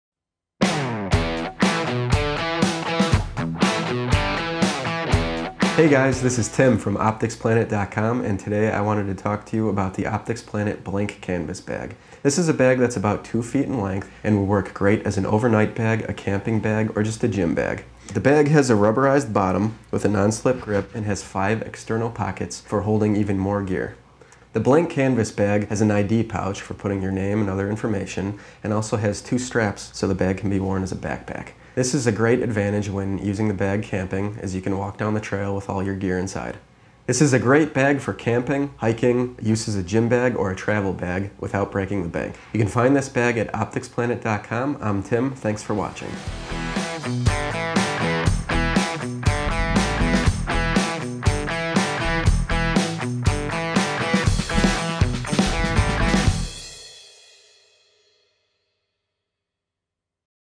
OpticsPlanet Blank Canvas Bag - OpticsPlanet.com Product in Focus
Get the OpticsPlanet Blank Canvas Bag now!

The OpticsPlanet blank canvas bag is a great bag for the gym, to go camping, or for a day bag and wont' break the bank. It's over 2 feet in length and has a huge interior. It also features a waterproof non-slip bottom, 5 exterior pockets to hold more gear, and an ID holder to properly keep track of your gear.

Another great feature of the OpticsPlanet blank canvas bag are two padded straps built to allow you to wear the bag on your back like a backpack, to keep you on the go. Check it out now!